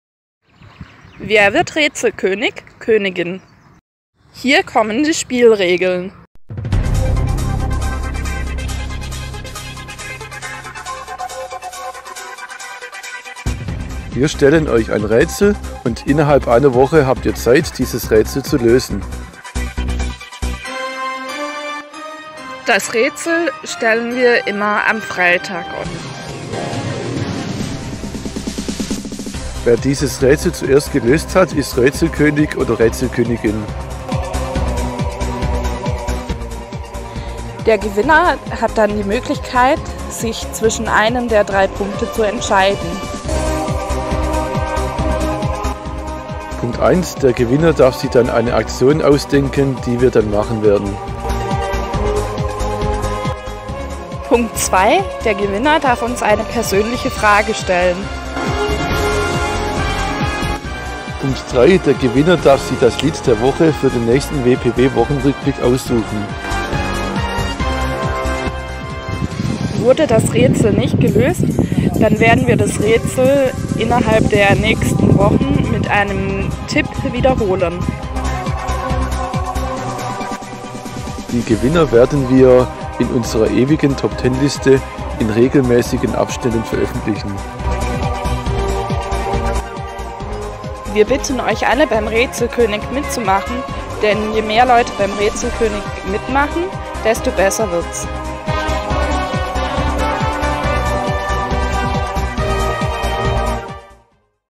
"Wer wird Rätselkönig/-in?" - die Spielregeln
Rätselkönig/-in
Am Freitag ist es endlich wieder soweit - wir suchen unsere/n nächste/n Rätselkönig/-in. In diesem Video erfährt ihr die Spielregeln.